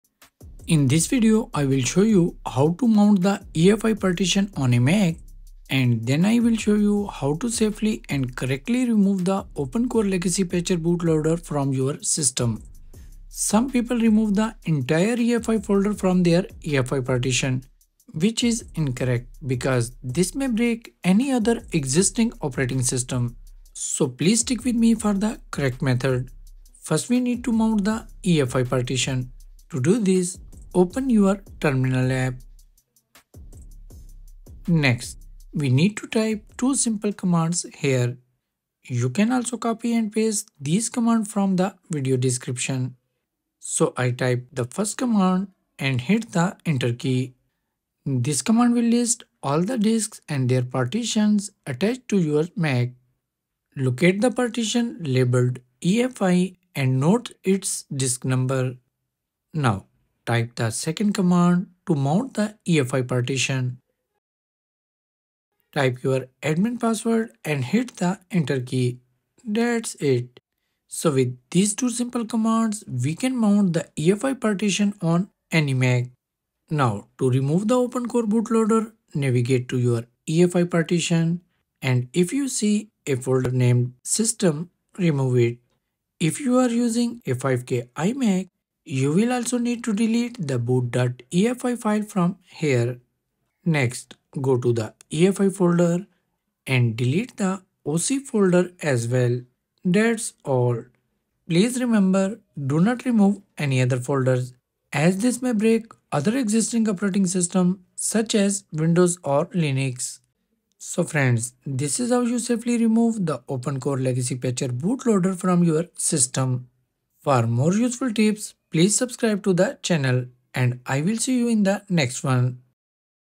How to mount EFI & remove OpenCore BootLoader Safely!
How to mount EFI and remove the OpenCore BootLoader Safely!  (Correct Method - 2024 )

Mac EFI Partition Commands
1st Command
diskutil list
2nd Command
sudo diskutil mount /dev/disk0s1

In this video, I will show you how to mount the EFI partition on a Mac, and then I will show you how to safely and correctly remove the OpenCore Legacy Patcher bootloader from your system.

Some people remove the entire EFI folder from their EFI partition, which is incorrect because  this may break any other existing operating system
so please stick with me for the correct method
First, we need to mount the EFI partition. To do this, open your terminal application.

Next, we need to type two simple commands here, You can also copy and paste these commands from the video description.

So I typed the first command and hit the enter key
This command will list all the disks and their partitions attached to your Mac.

Locate the partition labeled "EFI" and note its disk number.
Now,  type the second command to mount the EFI partition.
Type your admin password and Hit the enter key
That's it, So With these two simple commands, we can mount the EFI partition on any Mac.

Now, to remove the OpenCore boot loader, navigate to your EFI partition. If you see a folder named "System," remove it. If you’re using a 5K iMac, you’ll also need to delete the "boot.efi" file from here.

Next, go to the EFI folder and delete the "OC" folder as well. That's all! Do not remove any other folders, as this may break other existing operating systems, such as Windows or Linux.

So friends This is how you safely remove the OpenCore Legacy Patcher bootloader from your system.